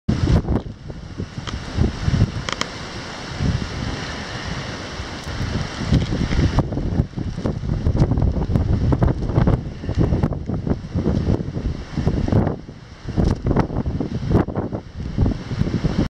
台風１９号 遊木：午後5時～
平成26年10月13日、午後５時過ぎの遊木漁港の様子です。